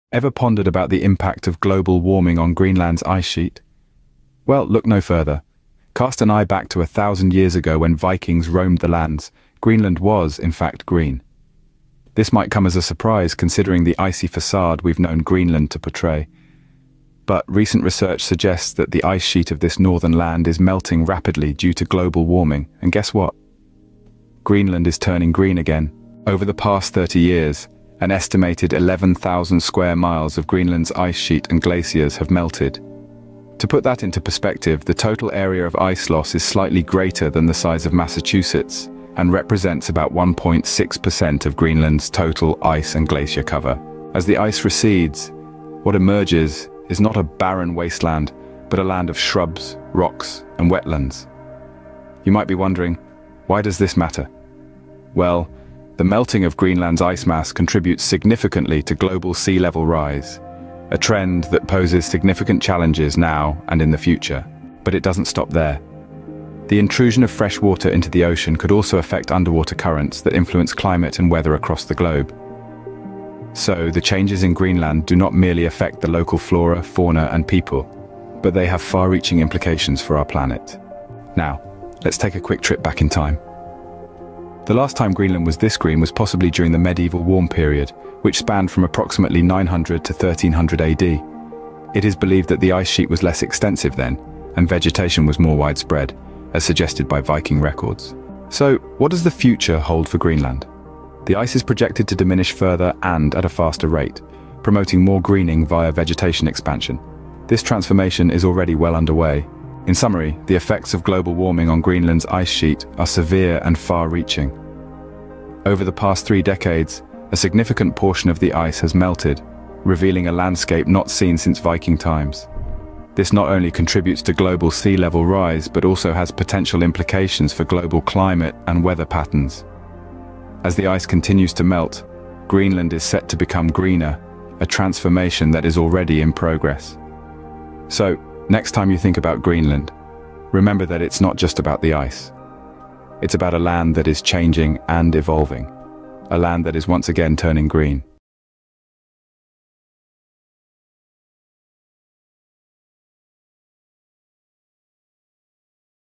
Greenland Transforms: Can We Stop It?
Join us on a gripping journey as we uncover the jaw-dropping effects of global warming on Greenland's pristine landscape. Want to delve deeper into the alarming reality of Greenland's melting ice?

The Greenland ice sheet, a critical component of our planet's climate system, is undergoing a dramatic metamorphosis. Melting glaciers and rising sea levels threaten coastal communities worldwide. This video explores the causes and consequences of Greenland's transformation.

Join us as we delve into the science behind ice melt, its impact on global sea levels, and potential solutions to slow this environmental crisis. Can we prevent Greenland from reaching a tipping point?

This video is perfect for you if you are interested in:

🟢 Climate change and its impact on glaciers
🟢 Rising sea levels and coastal flooding
🟢 The future of Greenland and its ice sheet
🟢 The urgency of climate action
🟢 Potential solutions for a sustainable future